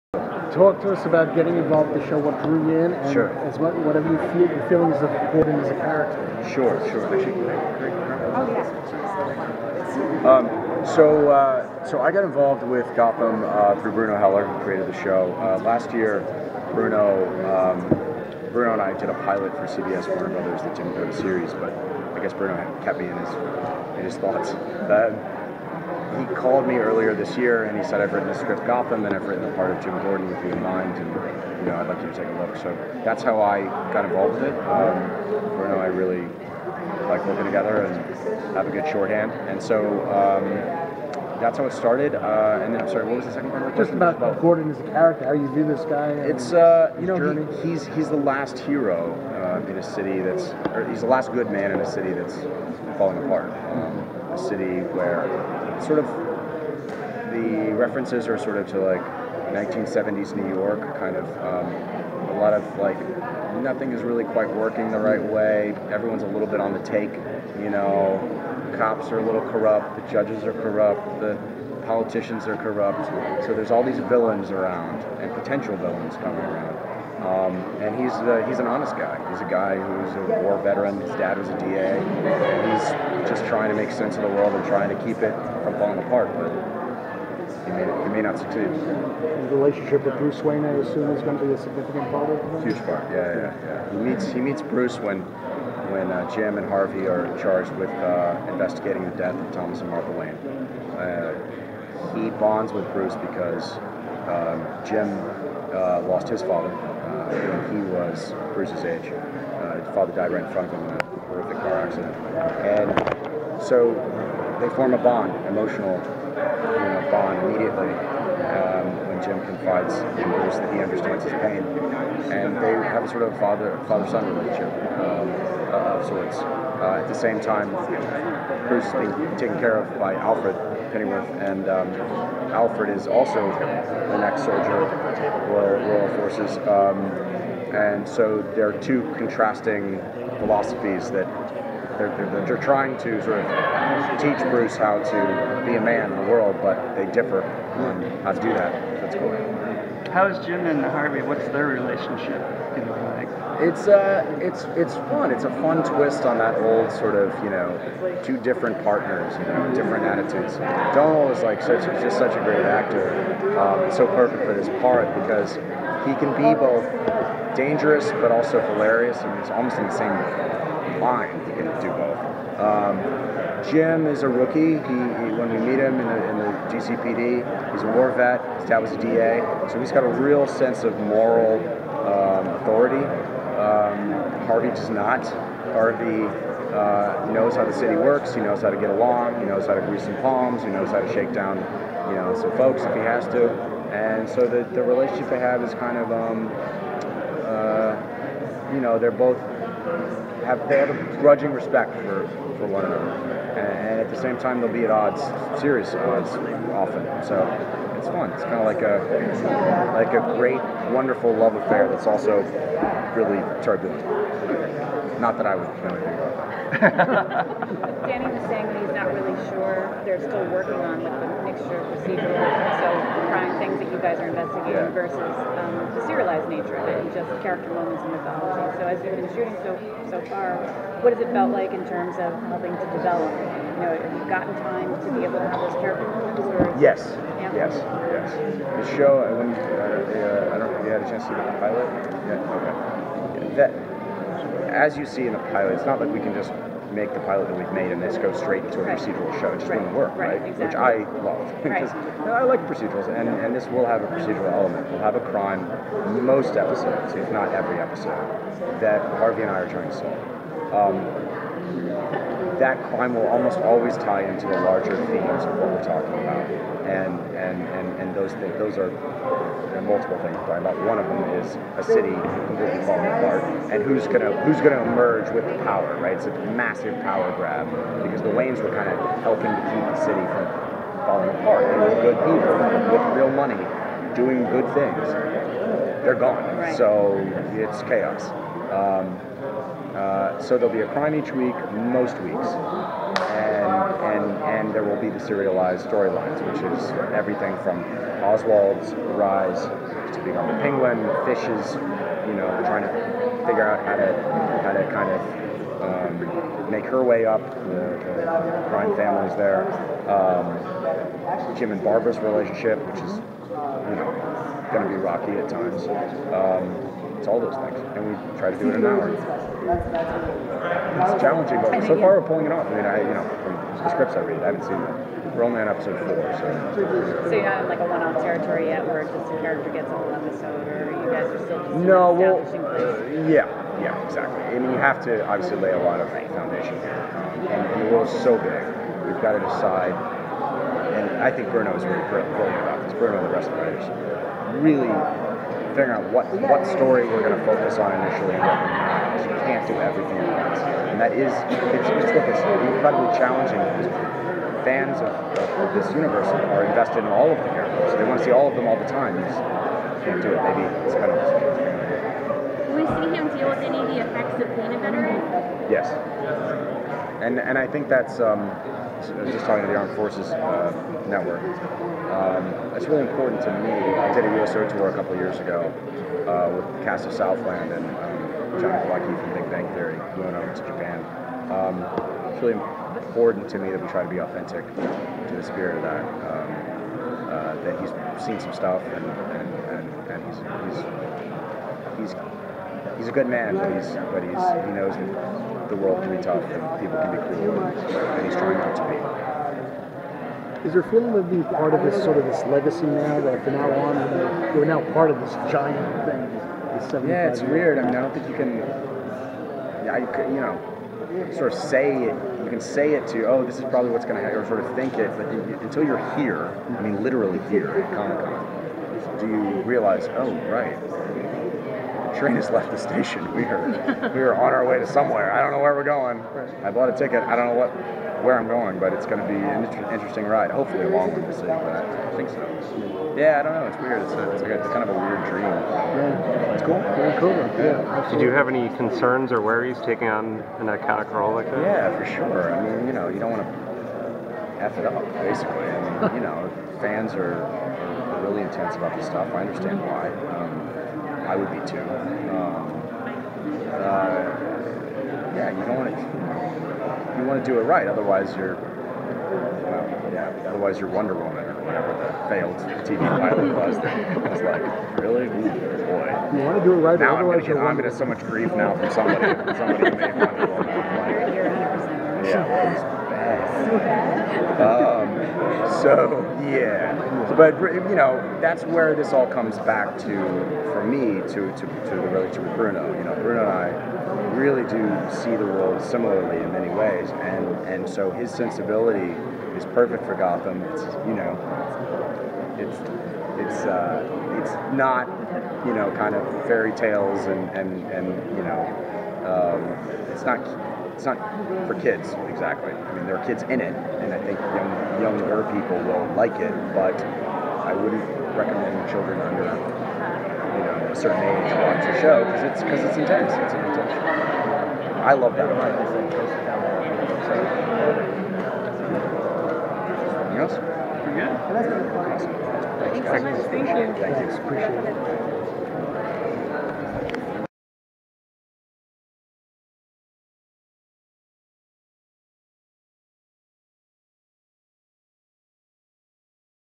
Gotham Q&A with star Ben McKenzie (SDCC 2014)
From the San Diego Comic-Con 2014 press room, GOTHAM star Ben McKenzie talks about playing Detective James Gordon on the new FOX series